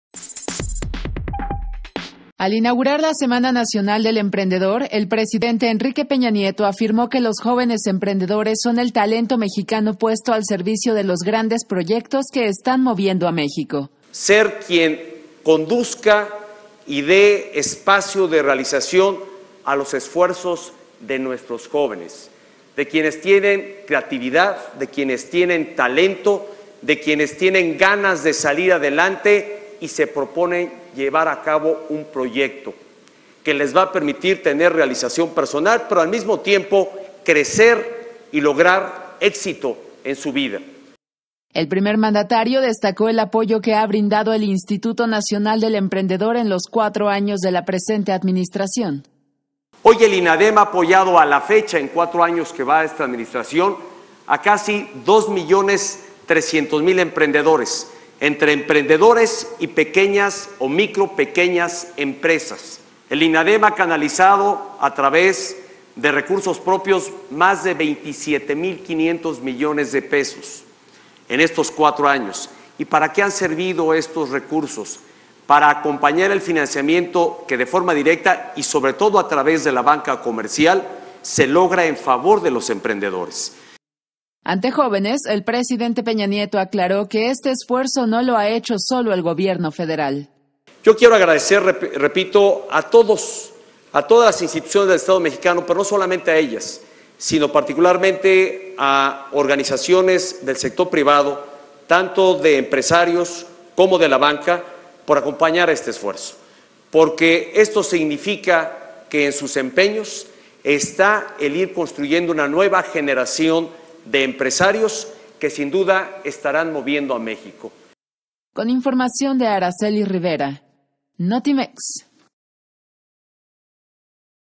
Jóvenes emprendedores el talento que mueve a Mexico: EPN
El Presidente de la República indicó que a la fecha INADEM ha apoyado a cerca de 2 millones 300 mil emprendedores